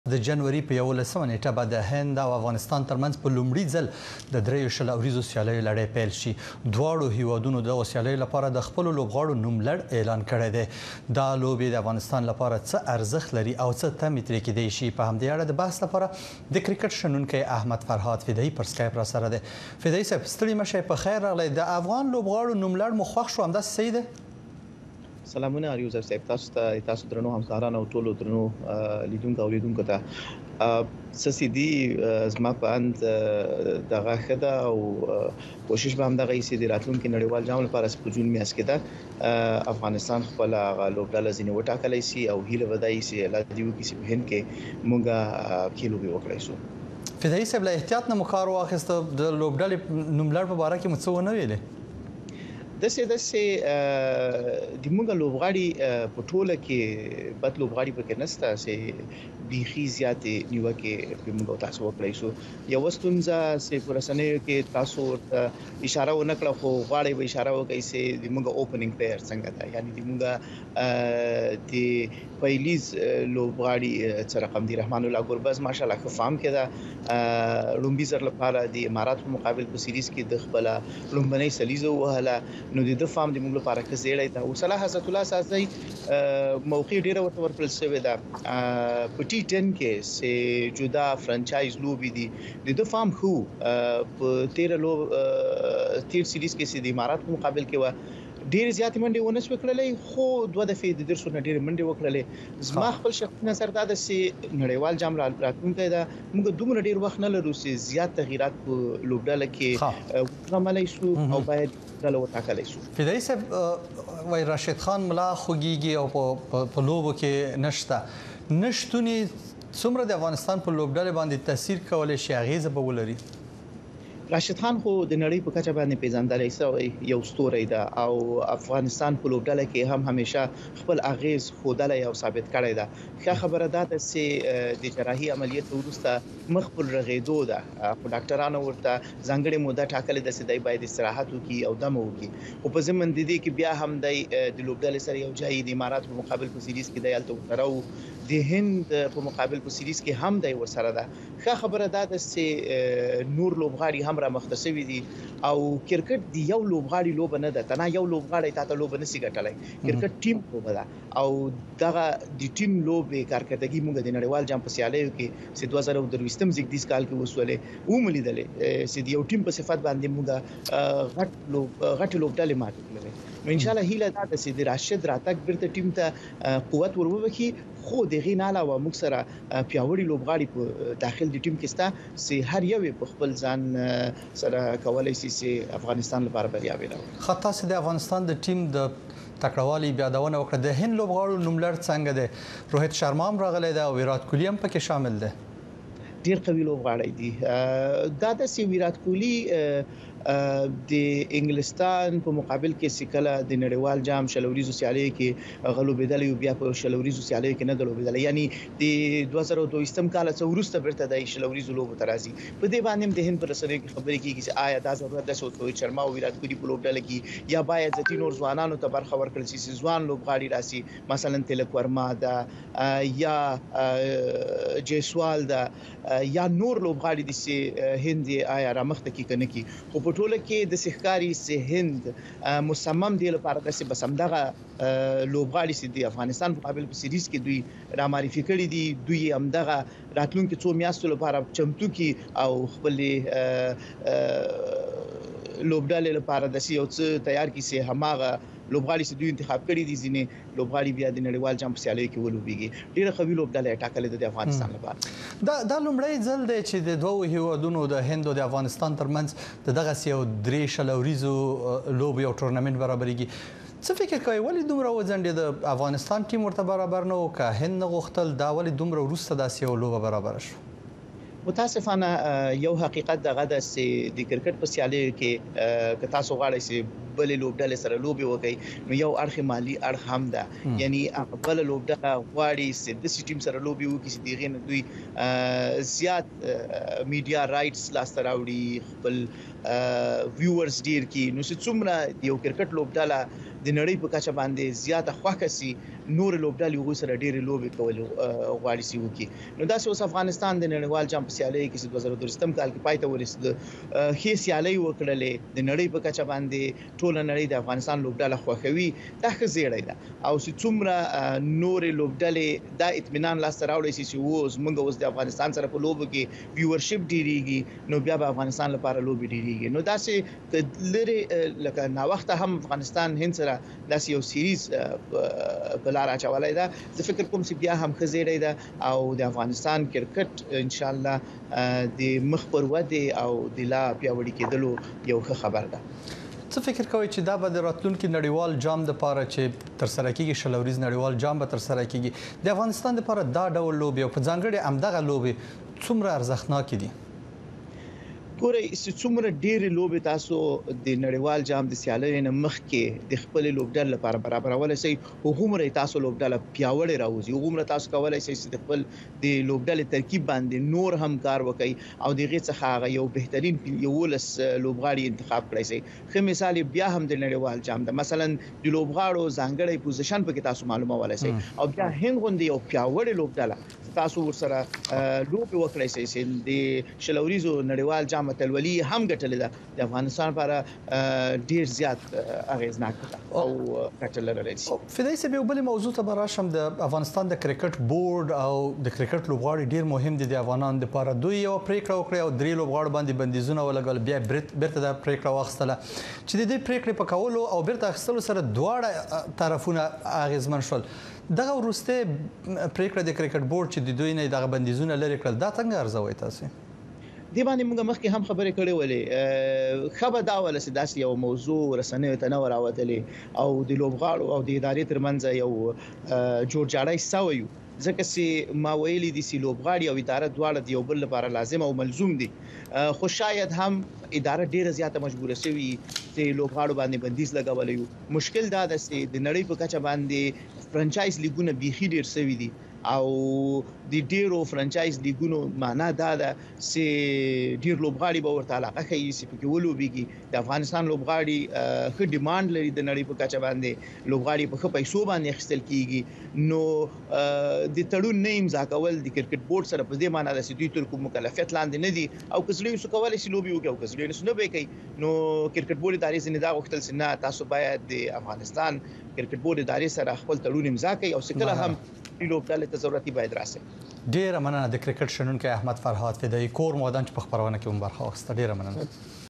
د هند کرکټ بورډ د افغانستان کرکټ لوبډلې پر وړاندې ډیره پیاوړې لوبډله اعلان کړې - فدايي
د کرکټ شنونکي احمد فرهاد فدايي د هند سره د افغانستان په شل اوریزو لوبو کې د راشد خان نشتون ته په اشارې وویل چې سره لدې چې راشد خان د کرکټ یو پیاوړی لوبغاړی دی، خو کرکټ ډلیزه لوبه ده او په دې لوبو کې هر لوبغاړی ارزښتناک دی. فدايي په دې باور دی چې د هند کرکټ بورډ د افغانستان د کرکټ لوبډلې پروړاندې ډیره پیاوړې لوبډله اعلان کړې ده.